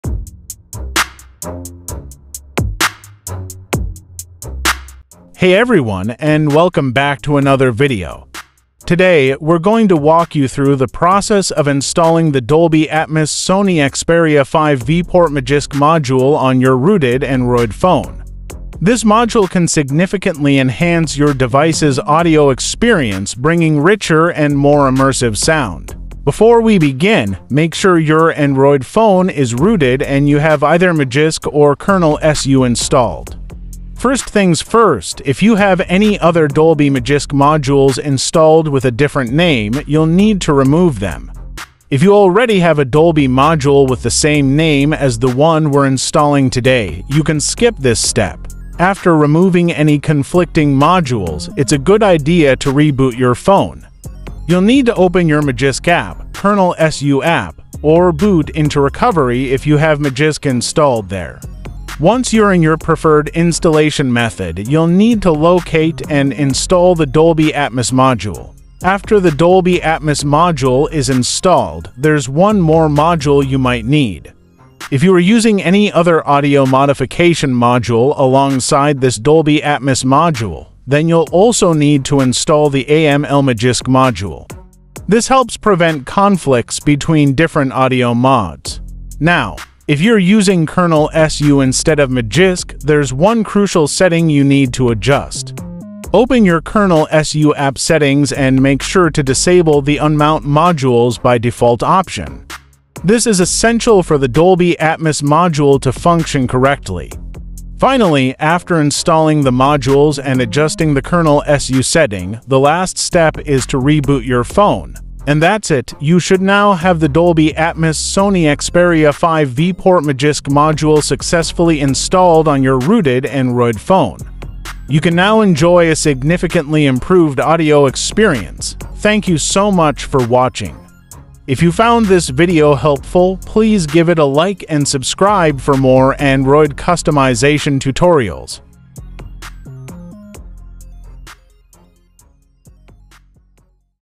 Install Dolby Atmos on ANY Rooted Android Phone! (Xperia 5 V Port)
🎧 Upgrade your Android phone's audio with the Dolby Atmos Sony Xperia 5 V Port Magisk Module! This video provides a step-by-step guide on how to install this fantastic sound enhancement on any rooted Android device using Magisk or KernelSU.

Don't miss out on enhancing your phone's audio quality. Watch now and experience the difference Dolby Atmos makes!